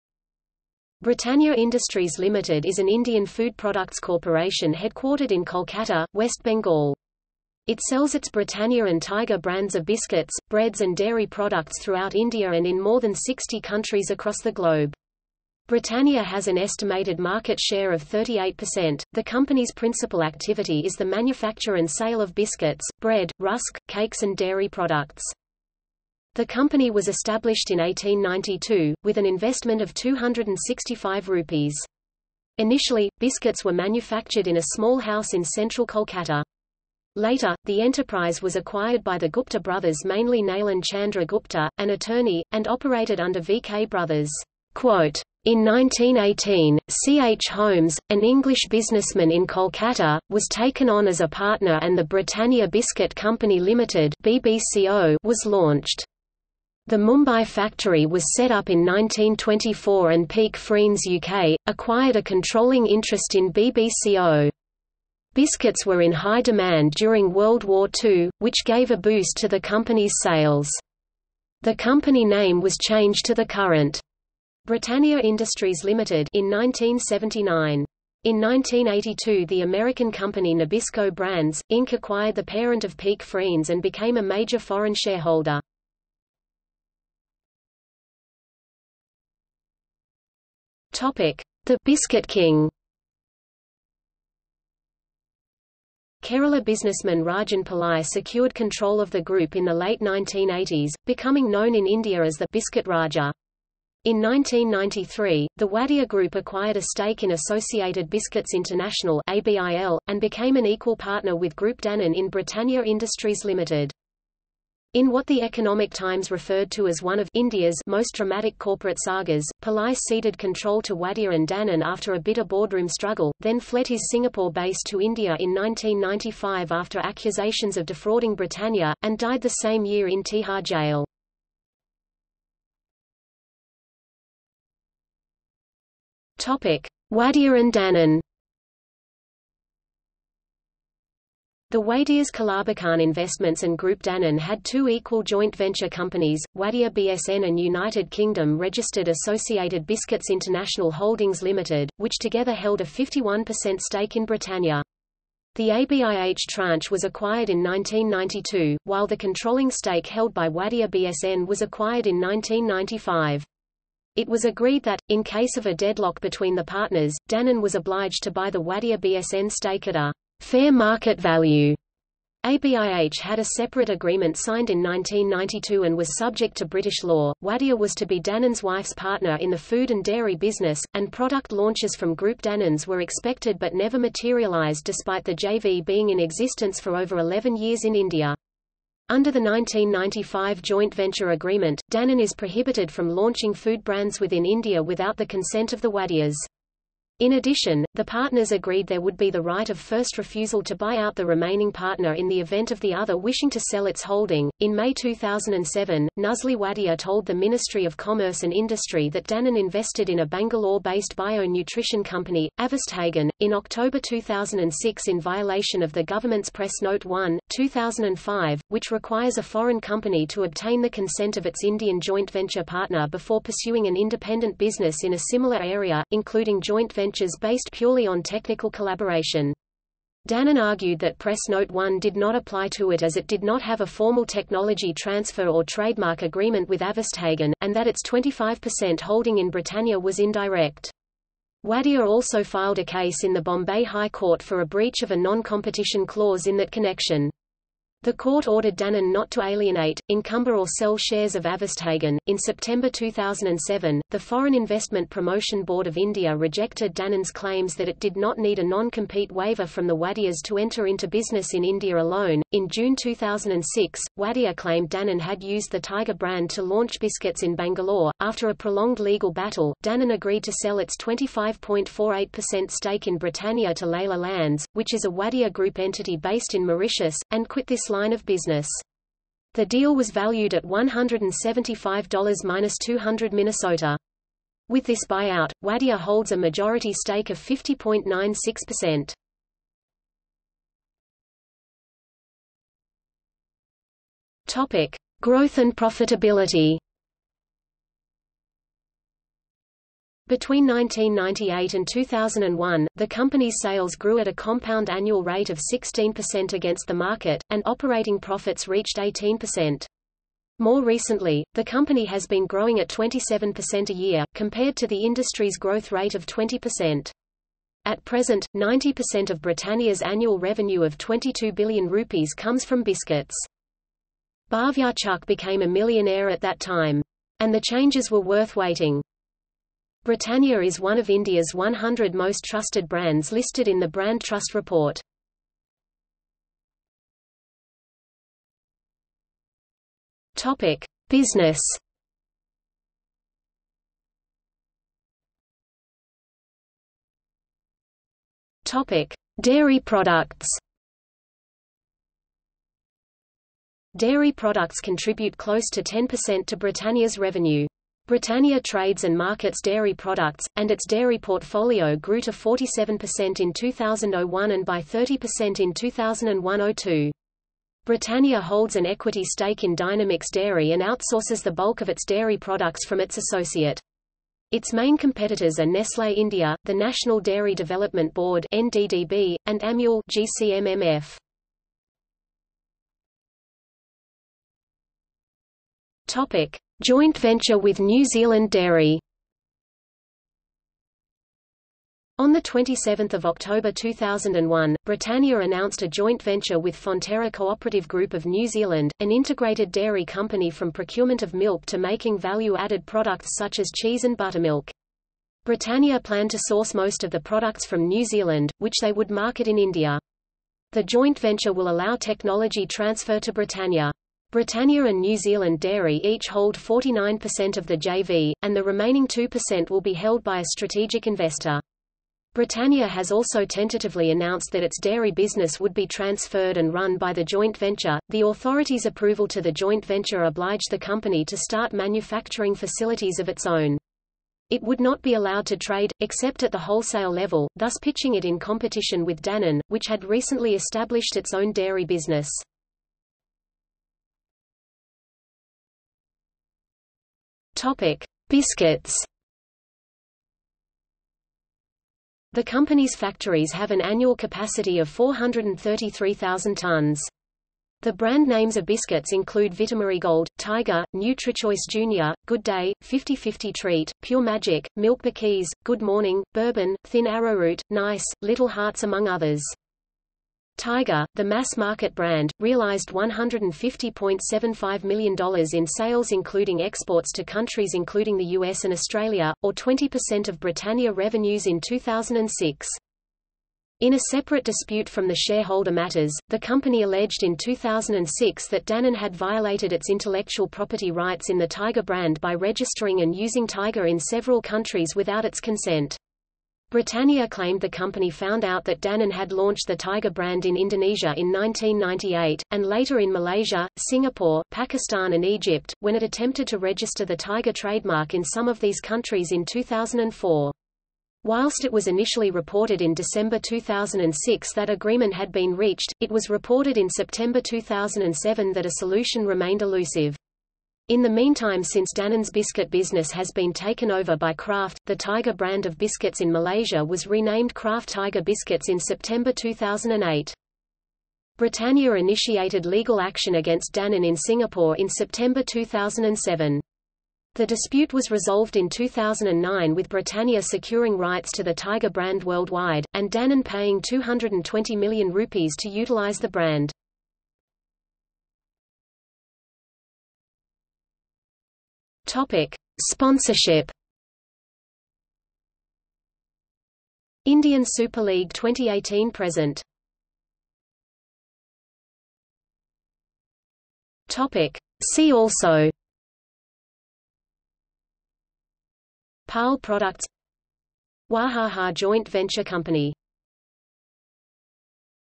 Britannia Industries | Wikipedia audio article
00:01:27 1 The 'Biscuit King'
00:02:12 2 Wadia and Danone
00:04:50 3 Growth and profitability
00:05:36 4 Business
00:05:45 4.1 Dairy products
00:06:23 4.1.1 Joint venture with New Zealand Dairy
00:07:24 4.2 Biscuits
00:09:08 5 Sponsorship
00:09:20 6 See also

- Socrates

SUMMARY
Britannia Industries Limited  is an Indian food-products corporation headquartered in Kolkata, West Bengal. It sells its Britannia and Tiger brands of biscuits, breads and dairy products throughout India and in more than 60 countries across the globe. Britannia has an estimated  market share of 38%.The company's principal activity is the manufacture and sale of biscuits, bread, rusk, cakes and dairy products.
The company was established in 1892, with an investment of ₹265. Initially, biscuits were manufactured in a small house in central Kolkata. Later, the enterprise was acquired by the Gupta brothers mainly Nalin Chandra Gupta, an attorney, and operated under V.K Brothers." In 1918, C.H. Holmes, an English businessman in Kolkata, was taken on as a partner and The Britannia Biscuit Company Limited (BBCo) was launched. The Mumbai factory was set up in 1924 and Peek Freans UK, acquired a controlling interest in BBCo. Biscuits were in high demand during World War II, which gave a boost to the company’s sales. The company name was changed to the current "Britannia Industries Limited" in 1979. In 1982 the American company Nabisco Brands, Inc. acquired the parent of Peek Freans and became a major foreign shareholder.